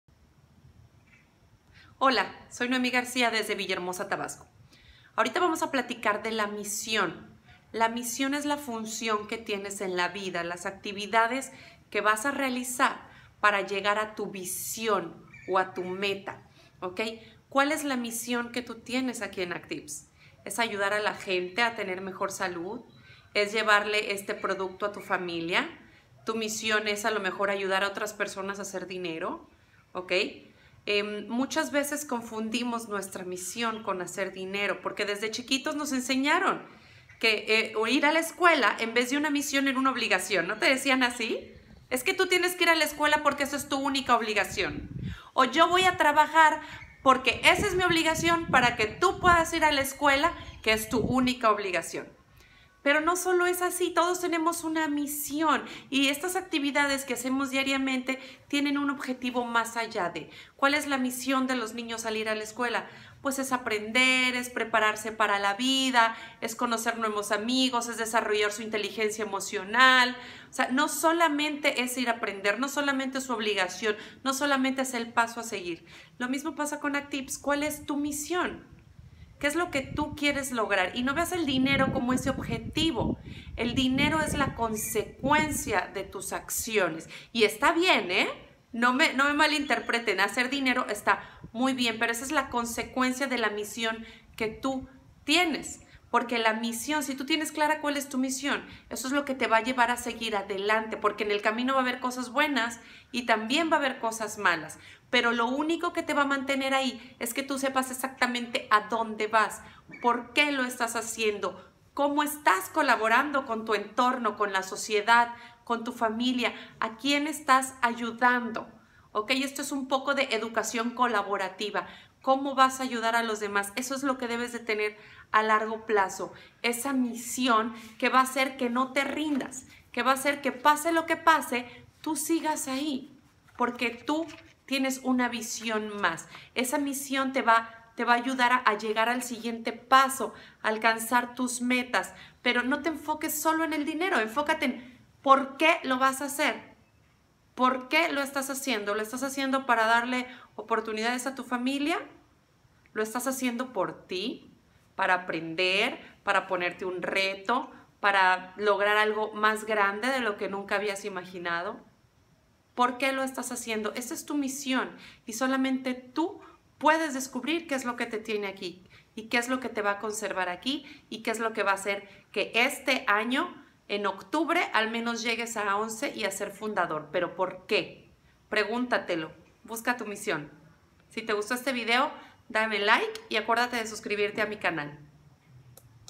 ACTIVZ y tu misión - GNM-X
ACTIVZ es tu misión

Sabes cuál es tu misión con ACTIVZ? Tu misión con ACTIVZ son las acciones que realizas cada día para llegar a un objetivo. Si no tienes clara cuál es tu misión con ACTIVZ no vas a saber cómo

¿Cuál es tu misión con ACTIVZ? ¿Es ayudar a las personas a mejorar su salud? ¿Mejorar su calidad de vida? ¿es llevarle este producto a tu familia? ¿Tu misión con ACTIVZ es ayudar a otros a hacer dinero?

Muchas veces actuamos por obligación sin tener clara cuál es nuestra misión con ACTIVZ, y por lo tanto damos pasos que no siempre conducen a lo que queremos.

Descubre tu misión con ACTIVZ, ¿qué quieres lograr? Si me contestas que quieres ganar dinero tal vez es porque no has encontrado tu misión con ACTIVZ. Hacer dinero no es el objetivo sino la consecuencia de lo que estás haciendo.

Si encuentras y cumples tu misión con ACTIVZ, te darás cuenta que el dinero viene por añadidura. Si sólo quieres hacer dinero por hacerlo, no vas a poder superar los tragos amargos. En el camino encontramos cosas buenas y malas, pero si tú sabes cuál es tu misión con ACTIVZ no vas a rendirte, porque sabes que tienes algo más importante por lo cual quedarte en ACTIVZ.

Tu misión con ACTIVZ es lo que te va a permitir llegar al siguiente paso, alcanzar tus metas. No te enfoques en el dinero sino en porqué estás haciendo este negocio.

El único que puede descubrir tu misión con ACTIVZ eres tú.  Y eso es lo que te llevará a lograr un A11 antes de octubre y ganar el Bono Fundador.